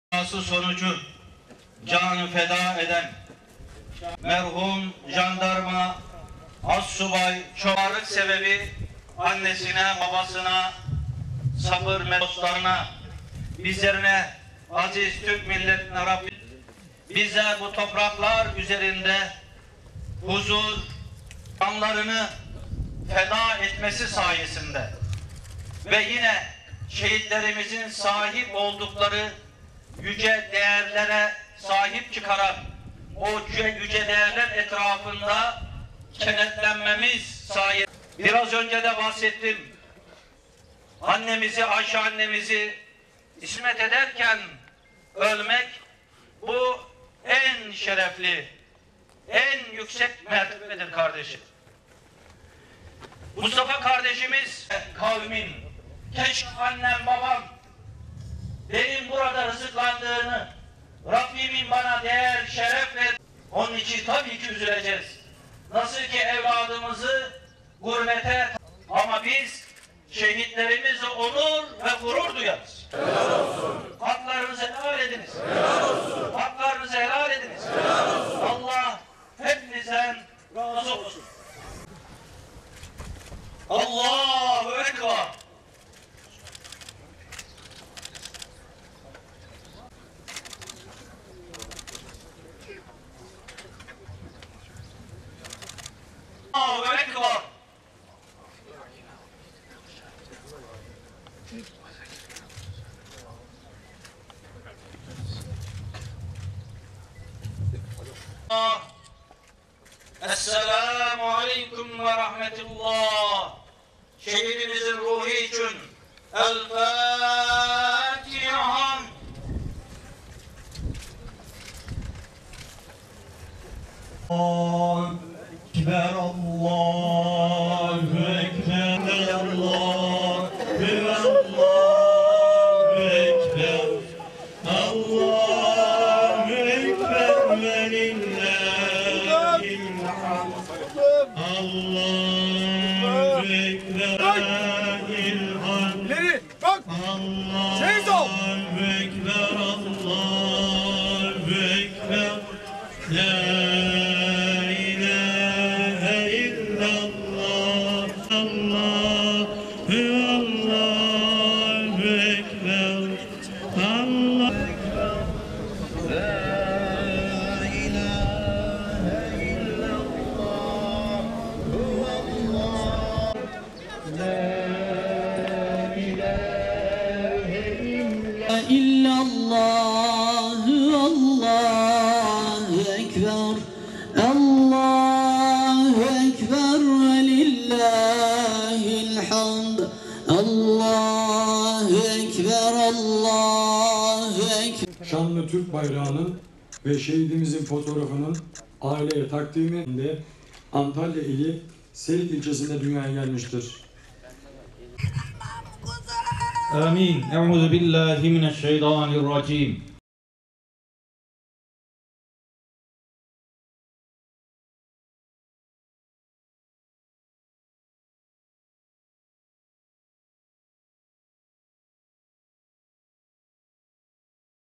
Şehit Mustafa Şen son yolculuğuna uğurlandı
Şırnak’ta devriye görevi sırasında meydana gelen trafik kazasında şehit olan Jandarma Asb. Çvş. Mustafa Şen’in (23) cenazesi memleketi Antalya’da defnedildi. Anne Ayşe Şen, “Kıyamadım, doyamadım bebeğim” diyerek gözyaşı dökerken, Baba İsmail Şen de şehidin tabutuna sarılıp ağladı.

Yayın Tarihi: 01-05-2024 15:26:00